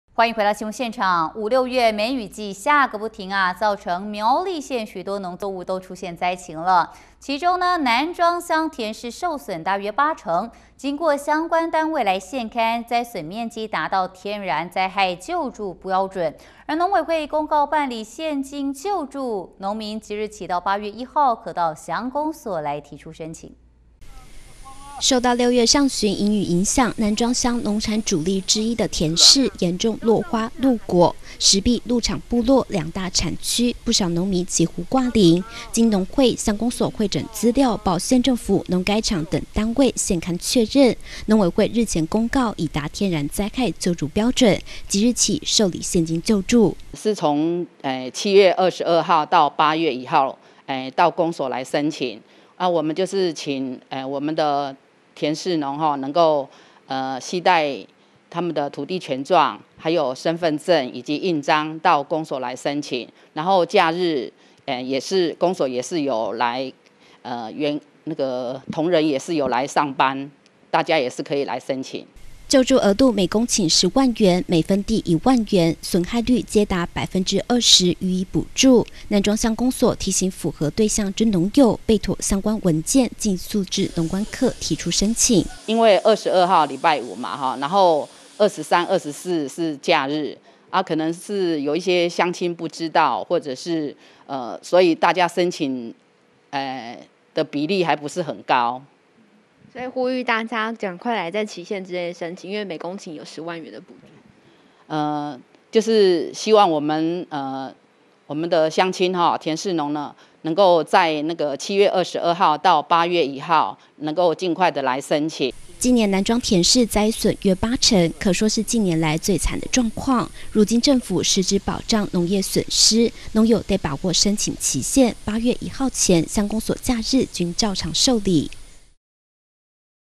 南庄甜柿受損約8成 開放申請現金救助│T-NEWS聯播網
五、六月梅雨季下不停，造成苗栗縣許多農作物出現災情，其中南庄鄉甜柿受損約八成，經相關單位現勘，災損面積達天然災害救助標準，農委會公告辦理現金救助，農民即日起至8/1，可至鄉公所提出申請。